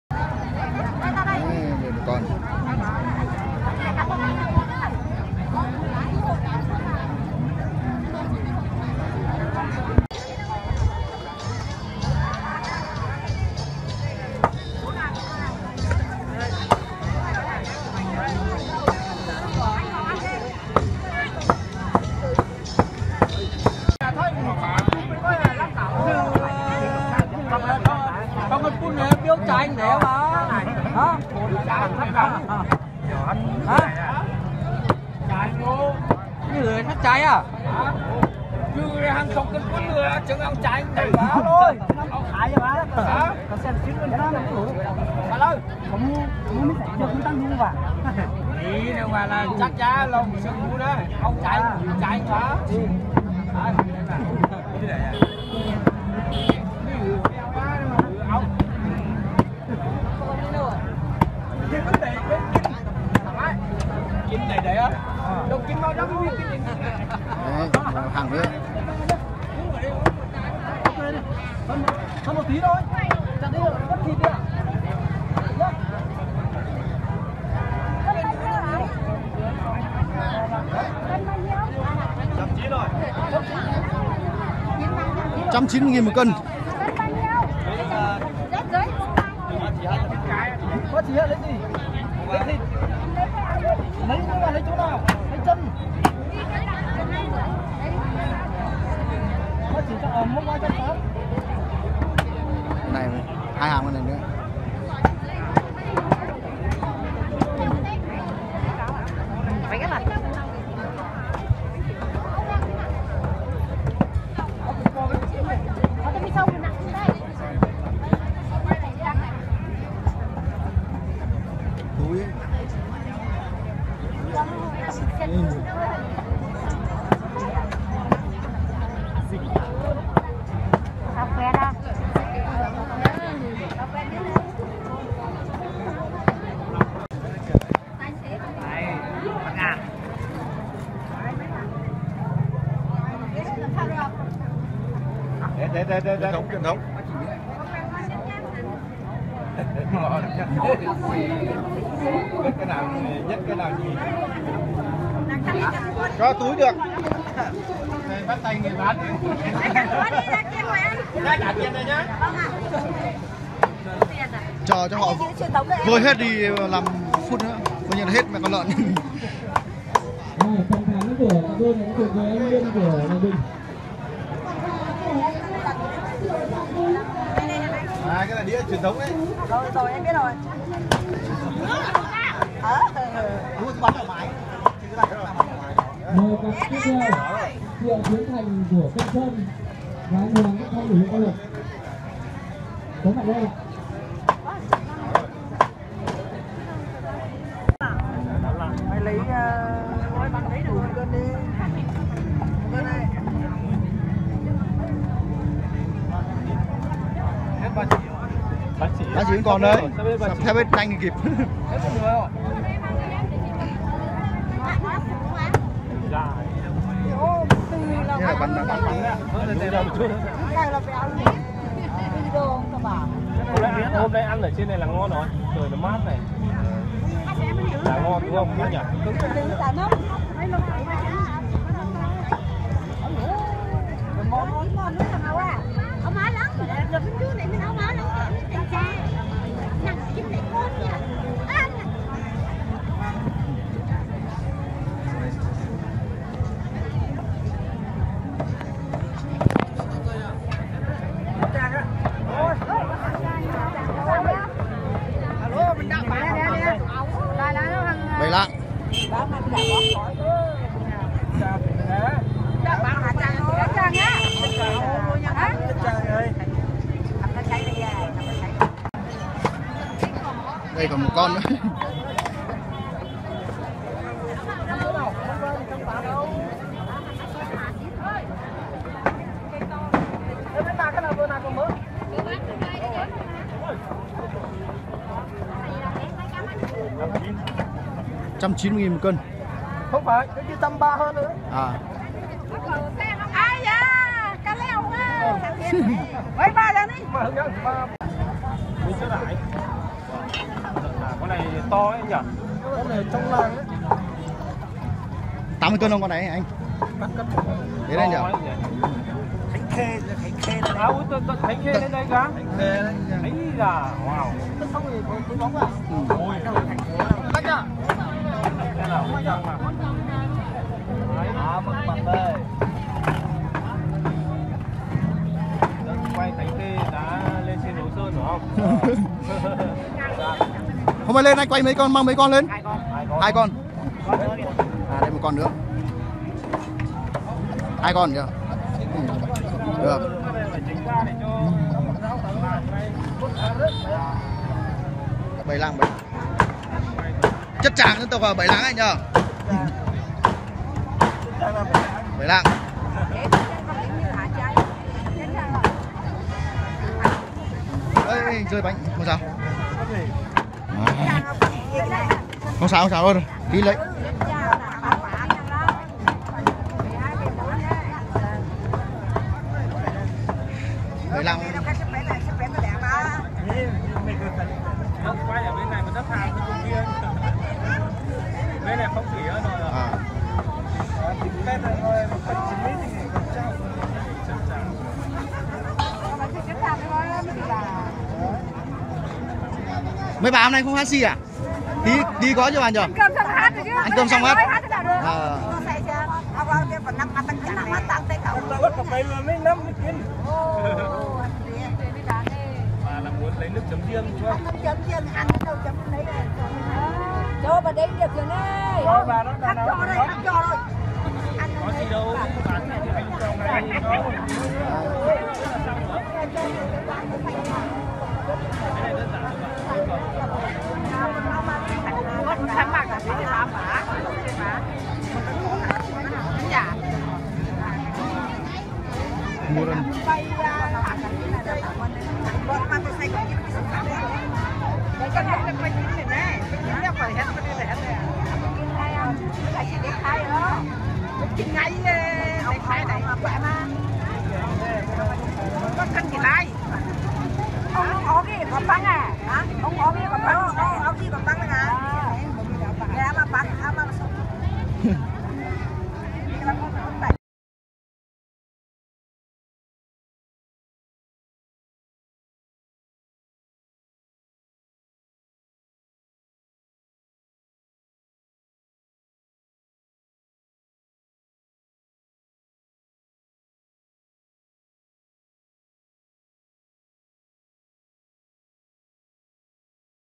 CAO THỦ NGƯỜI DAO MẪU SƠN TAY NGHỀ QUAY LỢN KHÔNG PHẢI DẠNG VỪA ĐÂU
MỜI CÁC BẠN XEM LỢN QUAY ĐẶC SẢN LẠNG SƠN